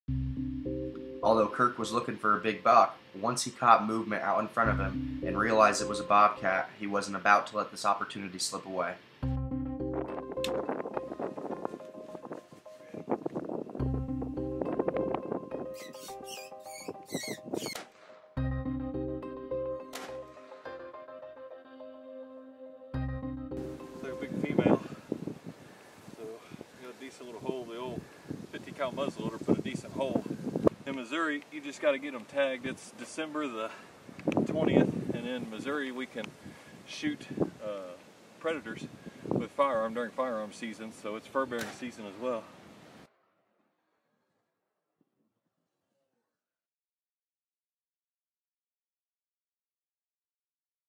Kirk's Bobcat Kill Shot
Kirk Hendershott was deer hunting in Northern Missouri when he was presented an opportunity at a bobcat.